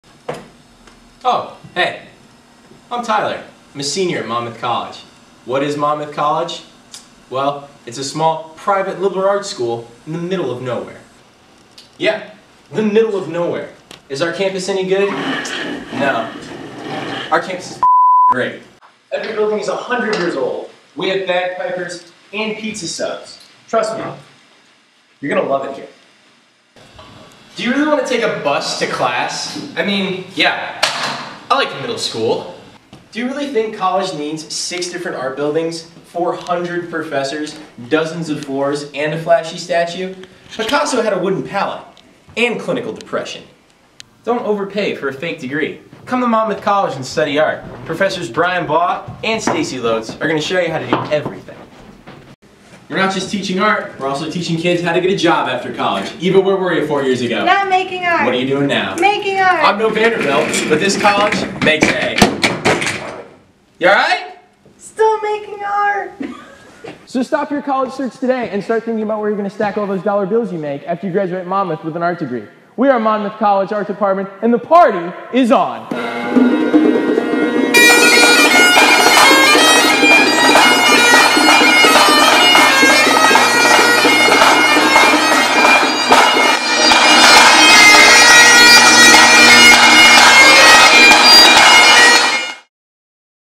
Monmouth College Art - Come Join The Party
An advertisement produced by Artisan Agency Advertising in conjunction with Red Door Enterprises Productions
Client: Monmouth College Art Department